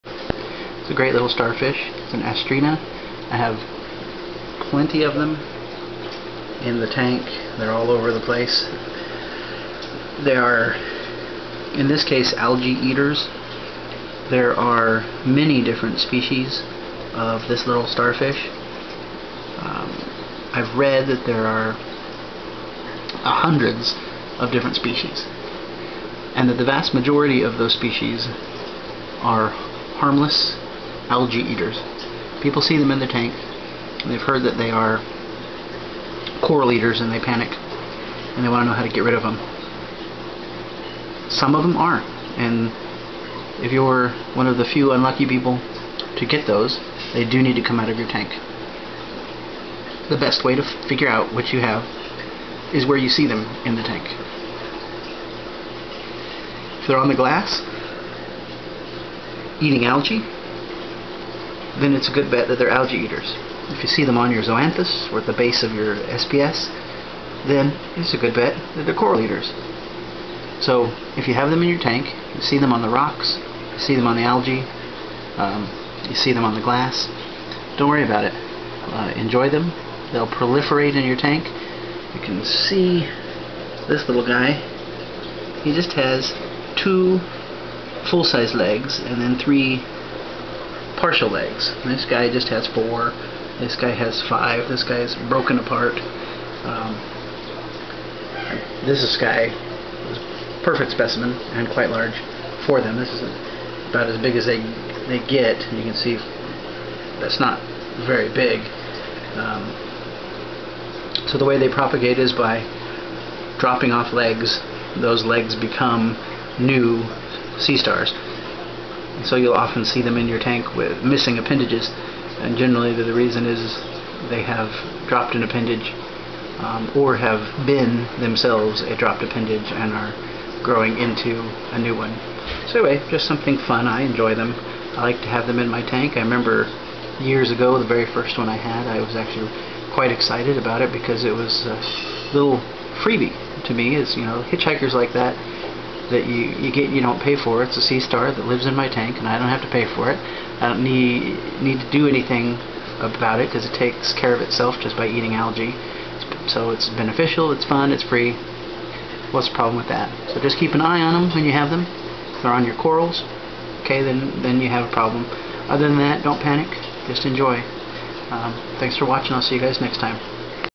Episode 32 - Asterina starfish: are they harmful?
Asterina starfish are common hitchhikers in reef tanks and they have a bad reputation as coral eaters. This video discusses if this reputation is deserved or not.